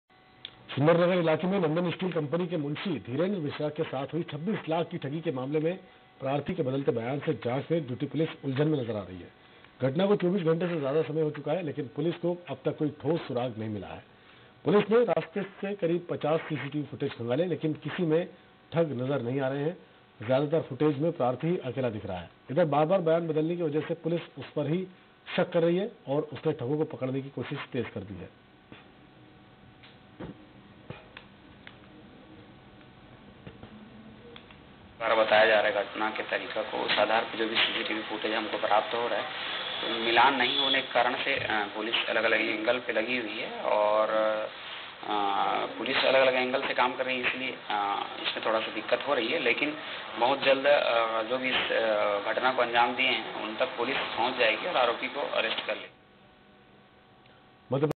Raipur News CG: नंदन स्टील कंपनी के मुंशी से धोखाधड़ी का मामला...
Raipur Police Chhattisgarh Police छत्तीसगढ़ पुलिस